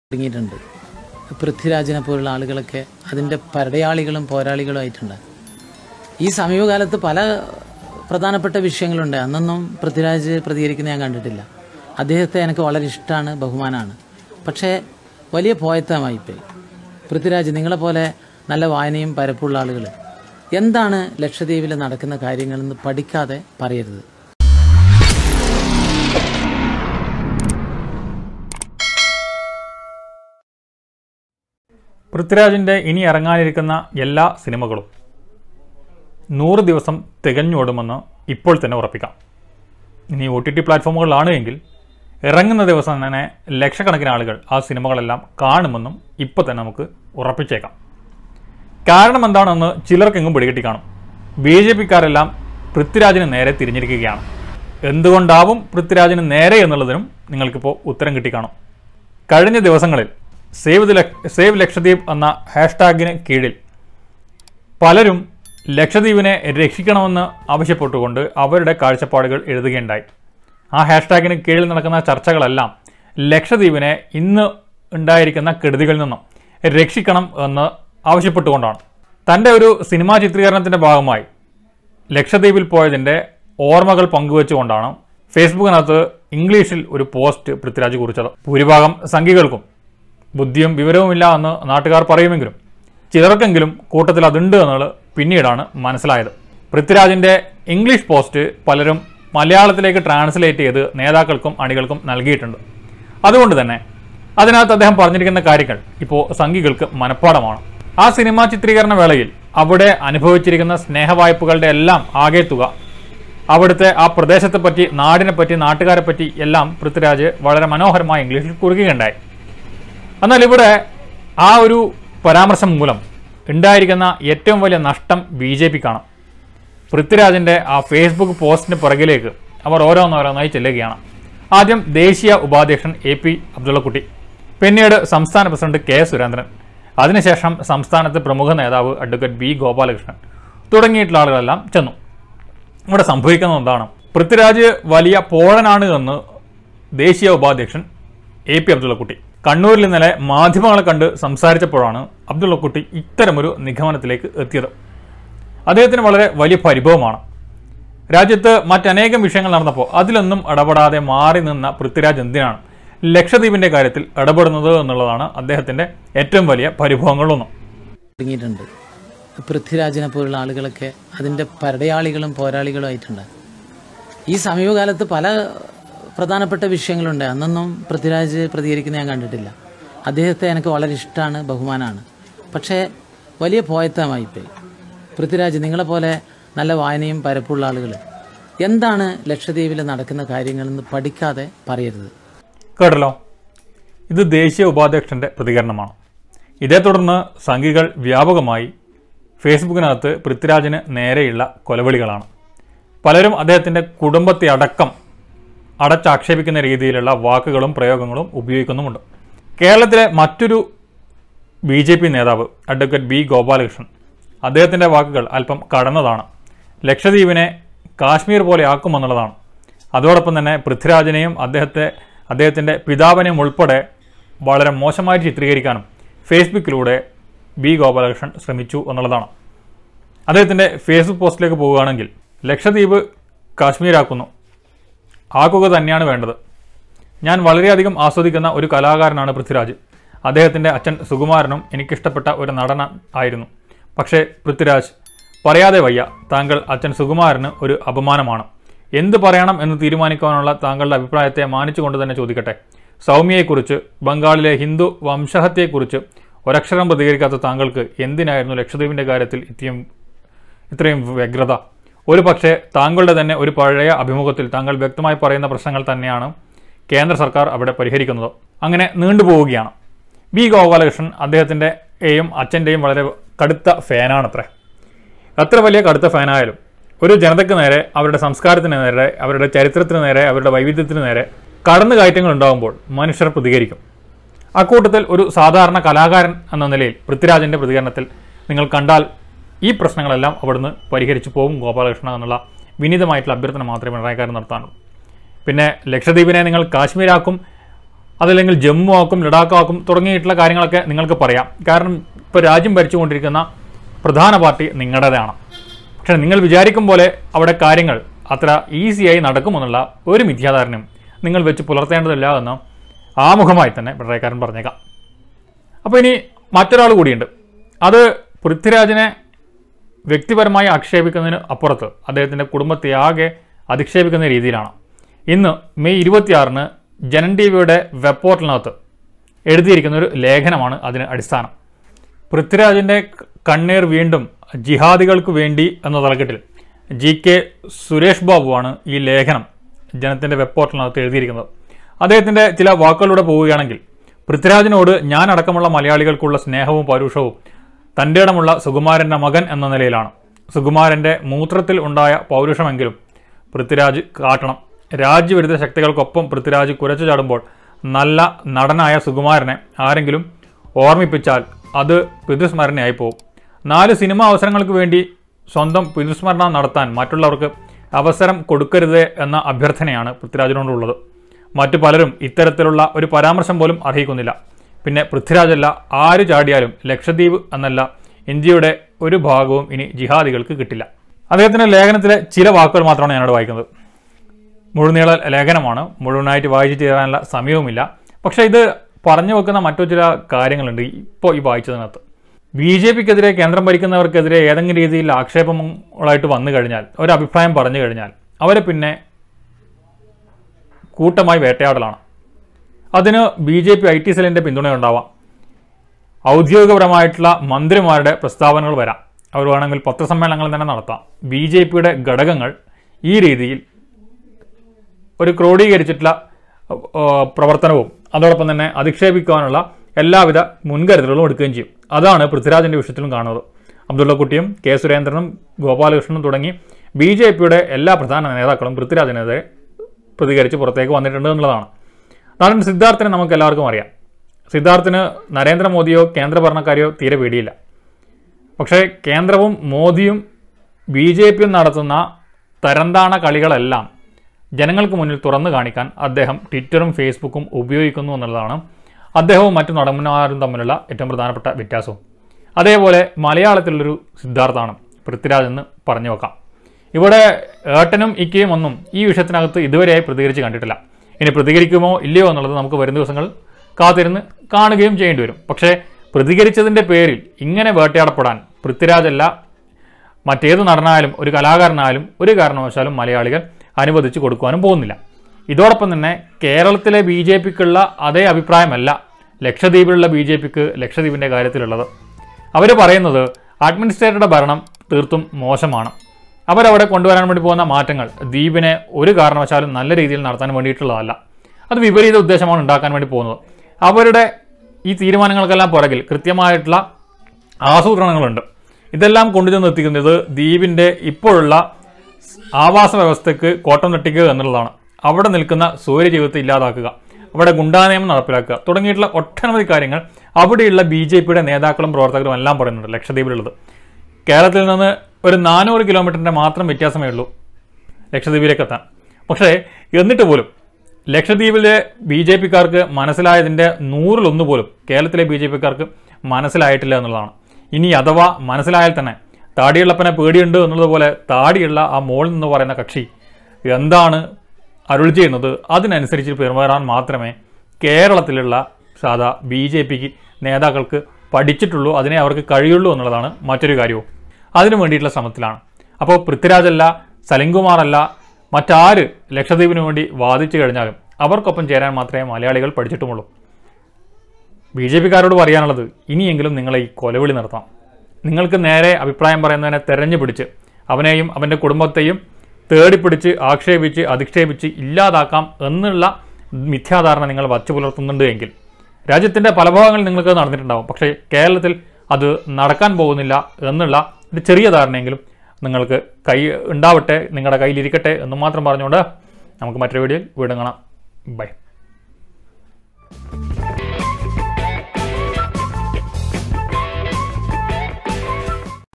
'സുകുമാരന്റെ മൂത്രത്തിലുണ്ടായ പൗരുഷമെങ്കിലും പൃഥ്വിരാജ് കാണിക്കണം | Prithviraj | Anarkali | Janam TV
‘സുകുമാരന്റെ മൂത്രത്തിലുണ്ടായ പൗരുഷമെങ്കിലും പൃഥ്വിരാജ് കാണിക്കണം; പൃഥ്വിക്ക് പിന്നില്‍ ജിഹാദികള്‍’; കുടുംബത്തെയും ആക്ഷേപിച്ച് ജനംടിവി

ലക്ഷദ്വീപ് ജനതയെ പിന്തുണച്ചതിന് പിന്നാലെ നടന്‍ പൃഥ്വിരാജിനെയും കുടുംബത്തെയും ആക്ഷേപിച്ച് ജനം ടിവി ലേഖനം. സുകുമാരന്റെ മൂത്രത്തില്‍ ഉണ്ടായ പൗരുഷമെങ്കിലും പൃഥ്വിരാജ് കാണിക്കണമെന്നാണ് ജനം ടിവി ഓണ്‍ലൈനില്‍ പ്രസിദ്ധീകരിച്ച ലേഖനത്തിലെ പരാമര്‍ശം. പൃഥ്വിരാജിന്റെ കണ്ണീര്‍ വീണ്ടും ജിഹാദികള്‍ക്കു വേണ്ടി എന്ന തലക്കെട്ടില്‍ ജികെ സുരേഷ് ബാബുവാണ് ലേഖനം എഴുതിയിരിക്കുന്നത്. രാജ്യവിരുദ്ധ ശക്തികള്‍ക്കൊപ്പം പൃഥ്വിരാജ് കുരച്ചു ചാടുമ്പോള്‍ നല്ല നടനായ സുകുമാരനെ ആരെങ്കിലും ഓര്‍മ്മിപ്പിച്ചാല്‍ അത് പിതൃസ്മരണയായിപ്പോകുമെന്നും ലേഖനത്തില്‍ പറയുന്നു.

ലേഖനത്തിലെ പൃഥ്വിരാജിനെതിരായ പരാമര്‍ശങ്ങള്‍ ഇങ്ങനെ: ”ഇന്ന് ലക്ഷദ്വീപിനുവേണ്ടി പൃഥ്വിരാജ് കണ്ണീരൊഴുക്കി രംഗത്തു വരുമ്പോള്‍ അതിനു പിന്നില്‍ ജിഹാദികളുടെ കുരുമുളക് സ്‌പ്രേ ആണെന്ന് മനസ്സിലാക്കാന്‍ വലിയ പാണ്ഡിത്യമൊന്നും വേണ്ട. കഴിഞ്ഞ കുറച്ചുകാലമായി ജിഹാദികള്‍ക്കും ഭീകരര്‍ക്കും വേണ്ടി പൃഥ്വിരാജ് കണ്ണീരൊഴുക്കാന്‍ തുടങ്ങിയിട്ട്. ഒരു നടന്‍ എന്ന നിലയിലും അഭിനേതാവ് എന്ന നിലയിലും പൃഥ്വിരാജിനോട് സ്‌നേഹവും ആദരവുമുണ്ട്. ദേശീയകാര്യങ്ങളിലും ഭരണപരമായ കാര്യങ്ങളിലും പൃഥ്വിരാജ് കുരയ്ക്കുന്നത് ആര്‍ക്കുവേണ്ടിയാണെന്ന് ഇപ്പോള്‍ എല്ലാവര്‍ക്കും അറിയാം.”

”പൃഥ്വിരാജിനോട് ഞാന്‍ അടക്കമുള്ള മലയാളികള്‍ക്ക് ഉള്ള സ്‌നേഹം പൗരുഷവും തന്റേടവുമുള്ള സുകുമാരന്റെ മകന്‍ എന്ന നിലയിലാണ്. സുകുമാരന്റെ മൂത്രത്തില്‍ ഉണ്ടായ പൗരുഷമെങ്കിലും പൃഥ്വിരാജ് കാട്ടണം. രാജ്യവിരുദ്ധ ശക്തികള്‍ക്കൊപ്പം പൃഥ്വിരാജ് കുരച്ചു ചാടുമ്പോള്‍ നല്ല നടനായ സുകുമാരനെ ആരെങ്കിലും ഓര്‍മ്മിപ്പിച്ചാല്‍ അത് പിതൃസ്മരണയായിപ്പോകും. നാലു സിനിമാ അവസരങ്ങള്‍ക്കു വേണ്ടി സ്വന്തം പിതൃസ്മരണ നടത്താന്‍ മറ്റുള്ളവര്‍ക്ക് അവസരം കൊടുക്കരുതേ എന്ന അഭ്യര്‍ത്ഥനയാണ് പൃഥ്വിരാജിനോടുള്ളത്. മറ്റു പലരും ഇത്തരത്തിലുള്ള ഒരു പരാമര്‍ശം പോലും അര്‍ഹിക്കുന്നില്ല. പിന്നെ പൃഥ്വിരാജല്ല, ആര് ചാടിയാലും ലക്ഷദ്വീപ് എന്നല്ല, ഇന്ത്യയുടെ ഒരു ഭാഗവും ഇനി ജിഹാദികള്‍ക്ക് കിട്ടില്ല.”

കഴിഞ്ഞദിവസം ബിജെപി നേതാക്കള്‍ പൃഥ്വിരാജിനെതിരെ രംഗത്തെത്തിയിരുന്നു. പൃഥ്വിരാജ് അച്ഛന്‍ സുകുമാരന് അപമാനമാണെന്ന പ്രസ്താവനയാണ് ബിജെപി വക്താവ് കൂടിയായ ബി ഗോപാലകൃഷ്ണന്‍ നടത്തിയത്. അച്ഛന്റെ ഗുണങ്ങള്‍ അവശേഷിക്കുന്നുണ്ടെങ്കില്‍, വ്യക്തിത്വം കുറച്ചെങ്കിലും ബാക്കിയുണ്ടെങ്കില്‍ താങ്കളുടെ പോസ്റ്റിനെ പുനര്‍വിചിന്തനം ചെയ്യണമെന്നും ഗോപാലകൃഷ്ണന്‍ പൃഥ്വിരാജിനോട് ആവശ്യപ്പെട്ടു.

ഗോപാലകൃഷ്ണന്‍ പറഞ്ഞത്: ”ഞാന്‍ വളരെയധികം ആസ്വദിക്കുന്ന ഒരു കലാകാരനാണ് പൃഥ്വിരാജ്, അദ്ദേഹത്തിന്റെ അച്ഛന്‍ സുകുമാരനും എനിക്കിഷ്ടപ്പെട്ട ഒരു നടന്‍ ആയിരുന്നു. പക്ഷെ പൃഥ്വിരാജ്, പറയാതെ വയ്യ, താങ്കള്‍ അഛന്‍ സുകുമാരന് ഒരു അപമാനമാണ്. കൈപറ്റുന്ന പച്ചപ്പണത്തിനു ഉപരിയായി, കുറച്ചെങ്കിലും അച്ഛന്റെ ഗുണഗണങ്ങള്‍ താങ്കളില്‍ അവശേഷിക്കുന്നുണ്ടെങ്കില്‍, സൈനിക് സ്‌കൂളില്‍ നിന്നും താങ്കള്‍ നേടിയെടുത്ത വ്യക്തിത്വം കുറച്ചെങ്കിലും ബാക്കിയുണ്ടെങ്കില്‍ താങ്കള്‍ താങ്കളുടെ പോസ്റ്റിനെ പുനര്‍വിചിന്തനം ചെയ്യണം. പിന്നെ ലക്ഷദ്വീപിന്റേയും കാശ്മീരിന്റേയും ഗതി വിഗതികളും ഇന്ന് ഒരേ പോലെയാണ്, കാശ്മീരില്‍ പാക്കിസ്ഥാനി തീവ്രവാദികള്‍ ആണെങ്കില്‍ ലക്ഷദ്വീപില്‍ കട തിവ്രവാദികളുടെ സാന്നിദ്ധ്യം കണ്ടു തുടങ്ങി. കാശ്മീരില്‍ മഞ്ഞു മലകള്‍ ആയിരുന്നു മറയെങ്കില്‍, ലക്ഷദ്വീപില്‍ മഹാസമുദ്രം.

ലക്ഷദ്വീപ് വിഷയത്തില്‍ പൃഥ്വിരാജ് പറഞ്ഞത് പോഴത്തരമാണെന്ന് പറഞ്ഞ് ബിജെപി ദേശീയ ഉപാധ്യക്ഷന്‍ എ പി അബ്ദുള്ളക്കുട്ടിയും രംഗത്തെത്തിയിരുന്നു.
അബ്ദുള്ളക്കുട്ടി പറഞ്ഞത്: ”പൃഥ്വിരാജിനെ പോലെയുള്ള ആളുകള്‍ ലക്ഷദ്വീപ് വിഷയത്തിലെ പ്രചരണത്തിന്റെ പടയാളിയും പോരാളിയും ആയിരിക്കുകയാണ്. അടുത്തകാലത്ത് പല വിഷയങ്ങളും ഉണ്ടായിട്ടുണ്ട്. അന്നൊന്നും പൃഥ്വിരാജിനെ പ്രതികരിച്ച് കണ്ടിട്ടില്ല. അദ്ദേഹത്തെ എനിക്ക് ഇഷ്ടമാണ്, ബഹുമാനമാണ്. പക്ഷെ പറഞ്ഞത് വലിയ പോഴത്തരമായി പോയി. തീവ്രഗ്രൂപ്പുകളുടെ പ്രചരണത്തില്‍ പൃഥ്വിരാജിനെ പോലെയുള്ള പാവപ്പെട്ടവരും കുടുങ്ങി പോവുകയായിരുന്നു. ലക്ഷദ്വീപില്‍ എന്താണ് സംഭവിക്കുന്നതെന്ന് പഠിക്കാതെ പറയരുത്. ”